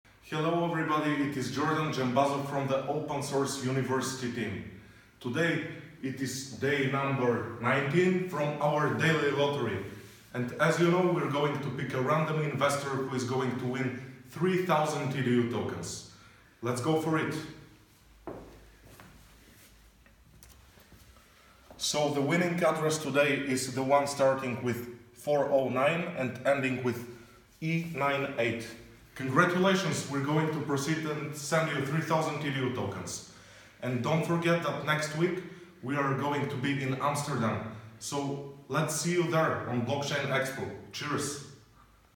OS.University Token Lottery | DAY 19
The OS.University token sale is live and we are announcing the first winner for the EDU token LOTTERY!

And the winner is: 0x409BAD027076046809Eaa6aD1Fb8bE710aC8Ee98 Congratulations!

Every 24 hours from the start of the ICO the Open Source University team will draw a random contributor’s address for that period and award it with additional 3,000 EDU.